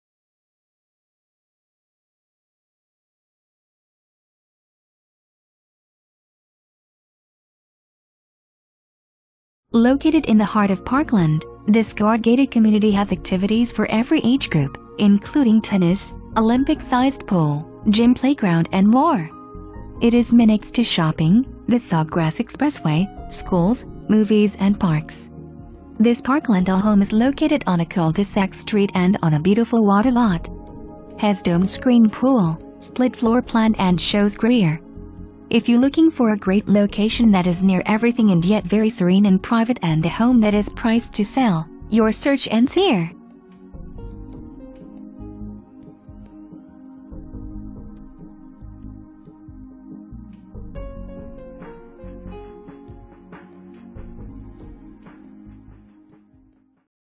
Parkland Florida Homes for Sale
4 bed, 3.0 bath, 2,145 SF

Located in the heart of Parkland, this guard gated community has activities for every age group, including tennis,olympic sized pool, gym playground and more!It is minutes to shopping, the sawgrass expressway, schools, movies and parks. This Parkland isle home is located on a cul-de-sac street and on a beautiful water lot. Has domed screened pool, split floor plan and shows grear! if you looking for a great location that is near everything and yet very serene and private and a home that is priced to sell, your search ends here!!